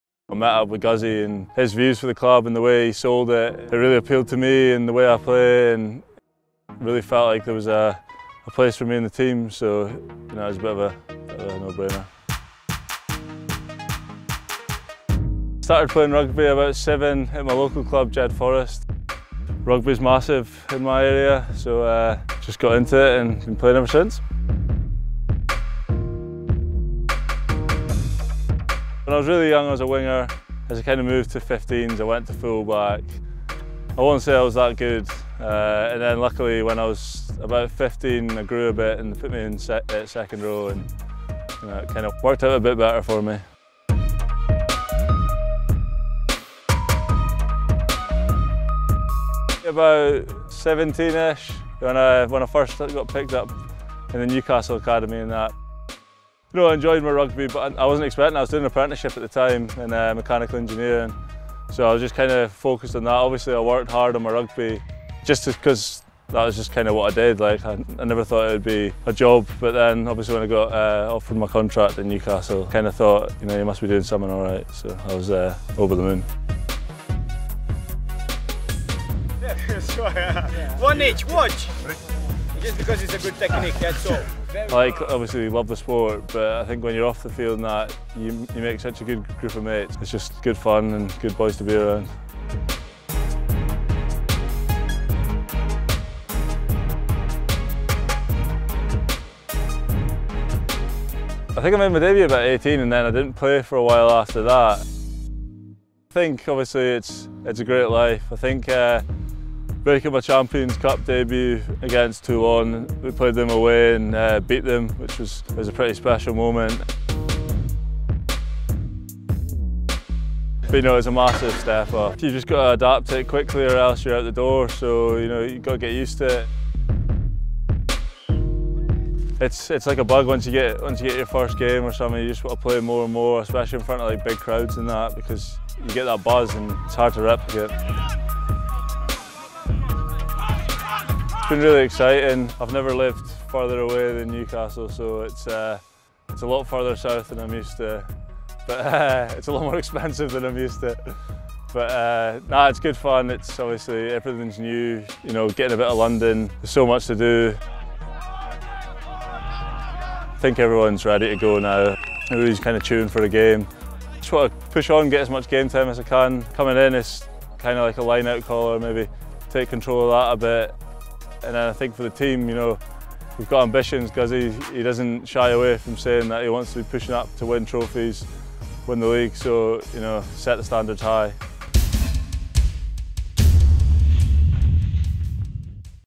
Meet Glen Young - Harlequins' new second row
Glen Young looks back on his journey in rugby which has taken him to The Stoop after he signed to play for Harlequins from this season.

The second row looks back on getting into rugby, his first experiences of playing and breaking into the Newcastle Falcons side.